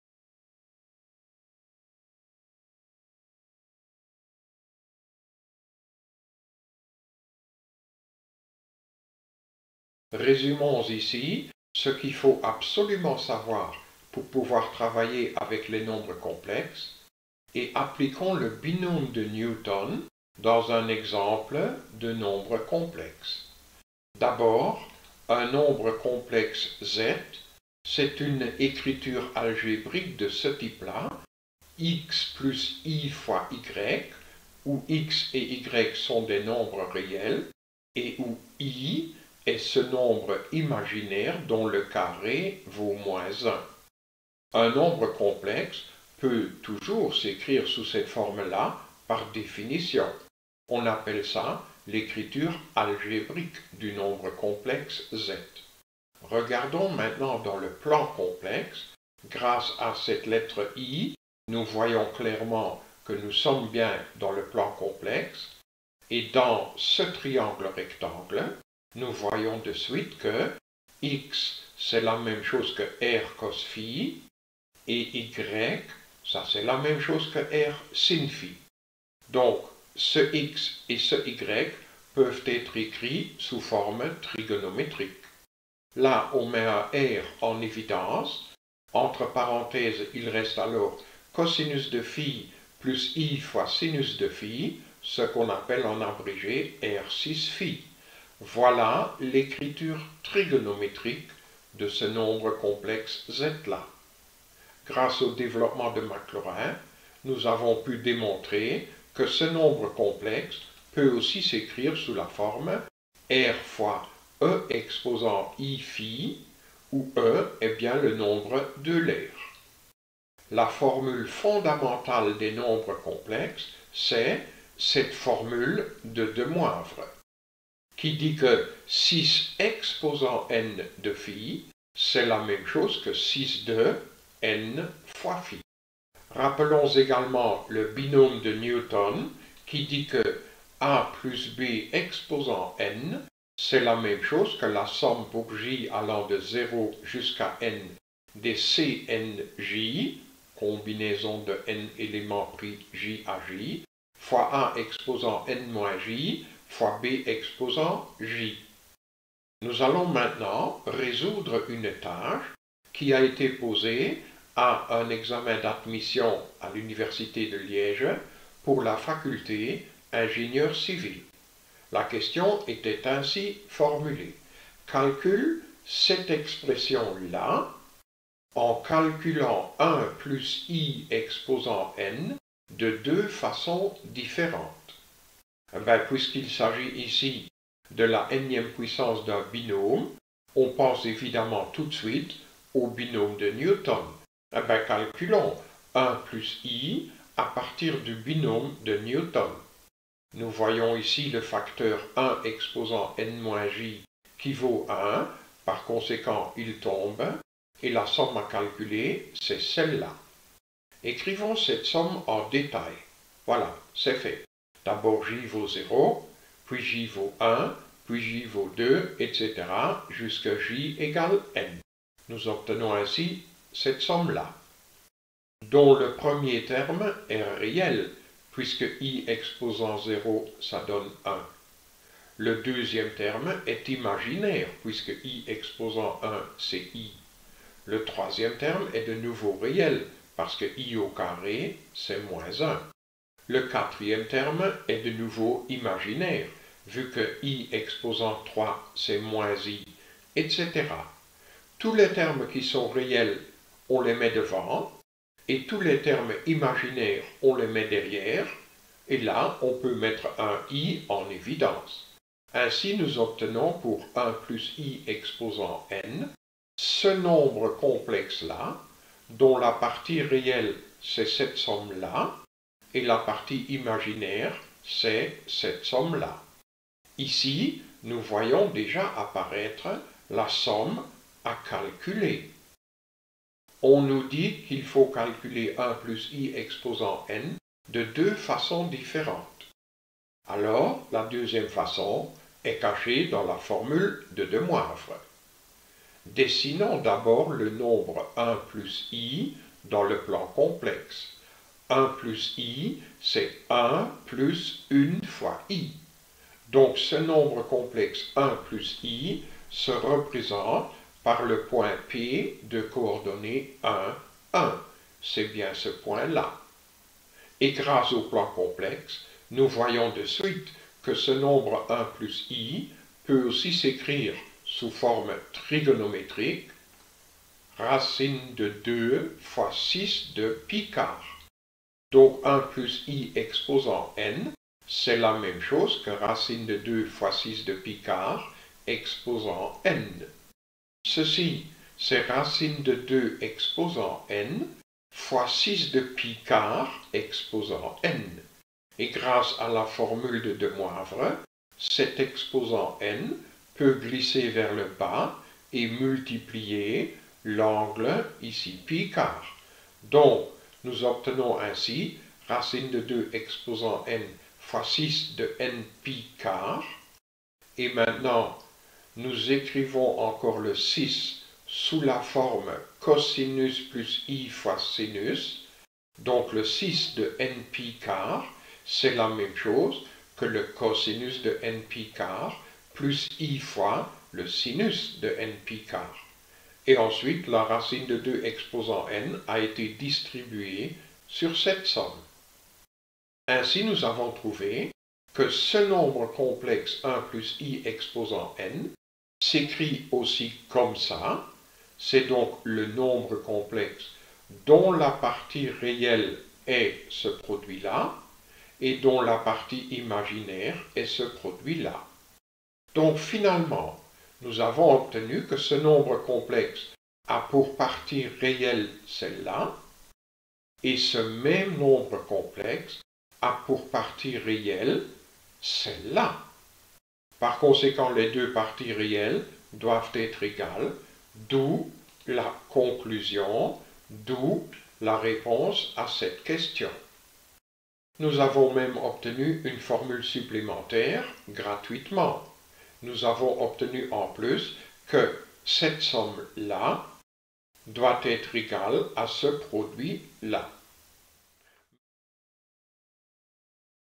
Les nombres complexes et le binôme de Newton F7014
F7014 Nous résumons ici les notions fondamentales des nombres complexes et nous créons un lien étrange entre l'analyse combinatoire et la trigonométrie en appliquant le binôme de Newton à un nombre complexe.